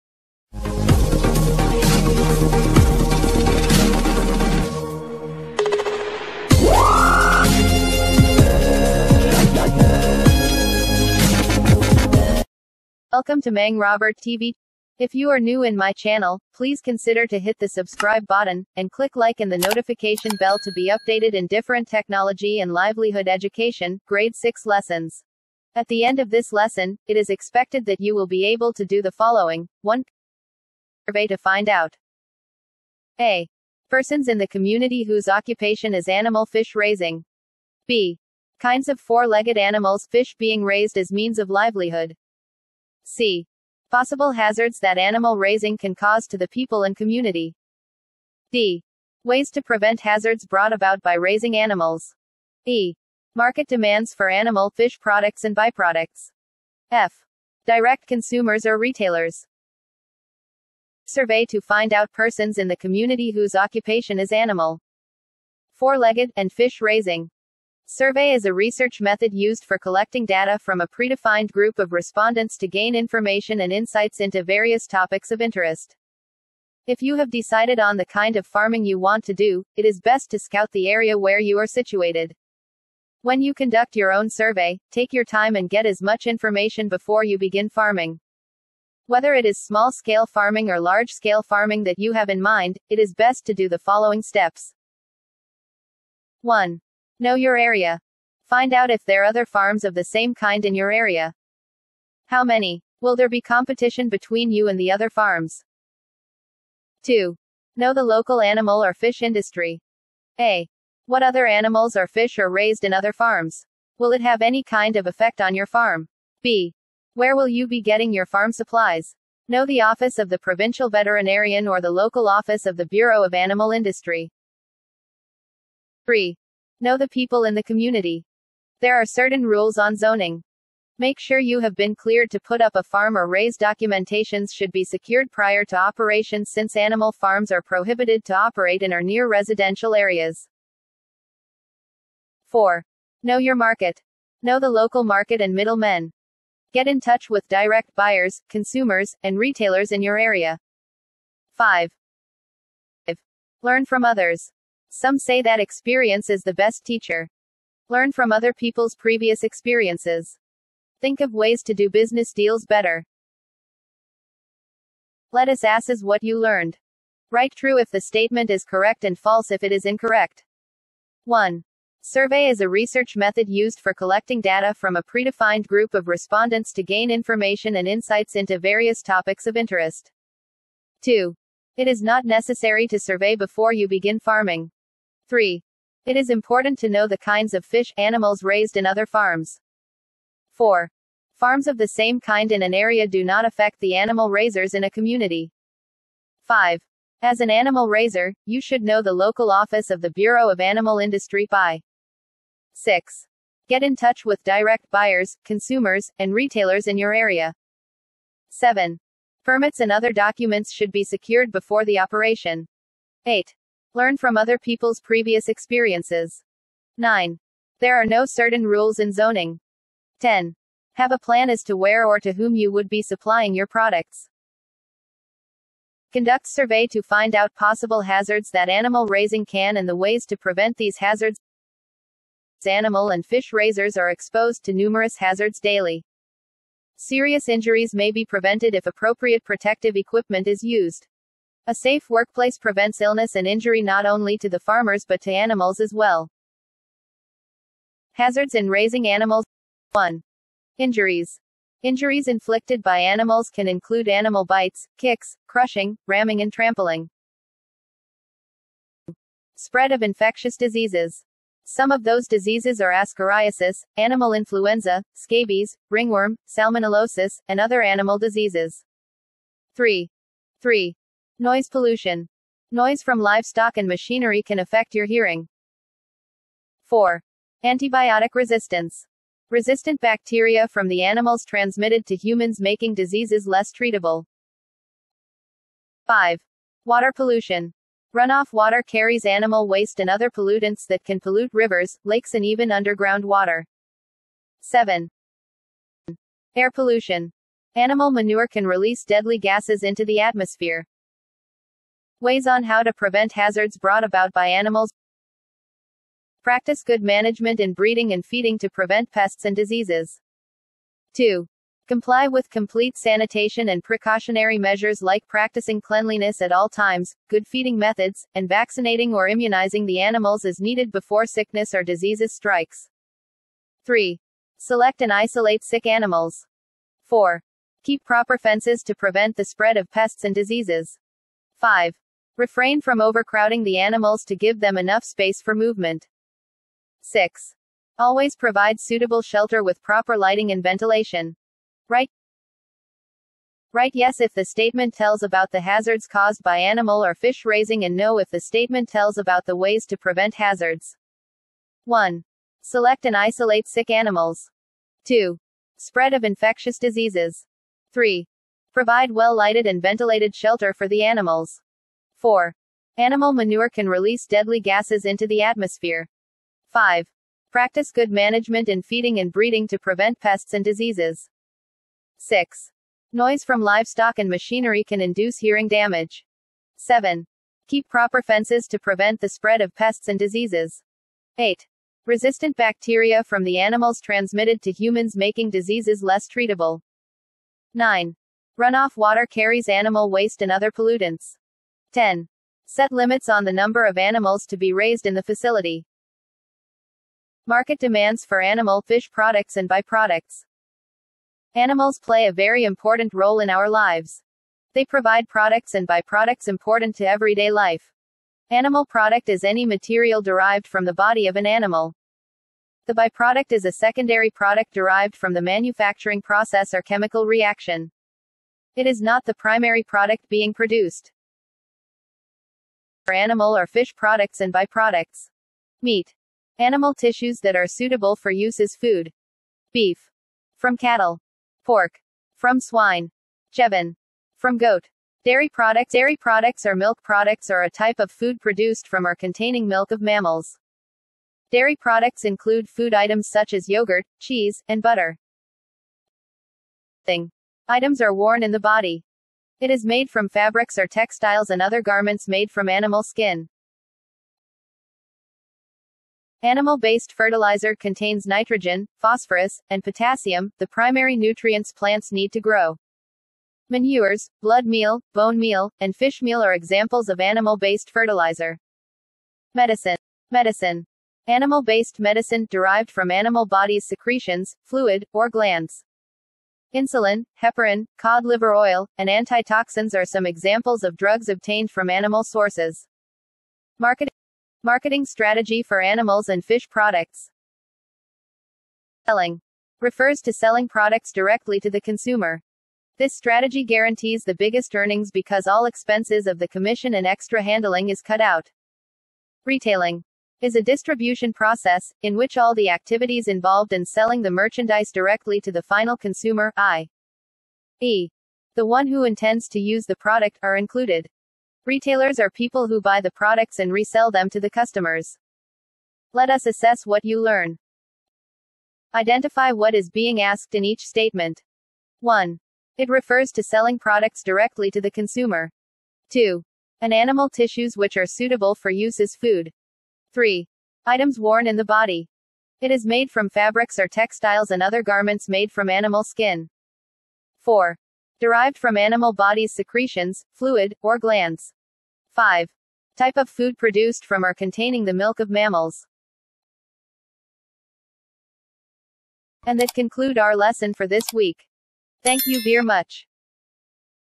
Conducts Survey | Module 7 | Grade 6| Agriculture | With assessment and answer key
At the end of this lesson, it is expected that you will be able to do the following:
1) conducts survey to find out.
a. persons in the community whose occupation is animal fish raising.
b. kinds of four-legged animals/fish being raised as means of livelihood.
c. possible hazards that animal raising can cause to the people and community.
d. ways to prevent hazards brought about by raising animals.
e. market demands for animal/fish products and byproducts.
f. direct consumers or retailers.

Answer key
Assessment no 1
1.True
2.False
3.True
4.True
5.True
6.True
7.True
8.True
9.false
10.True

Assessment no 2
1.No
2.Yes
3.No
4.Yes
5.No
6.Yes
7.No
8.Yes
9.Yes
10.No

Assessment no 3
1.Direct Selling
2.Meat
3.Clothing
4.Medecine
5.Dairy Product or Milk Product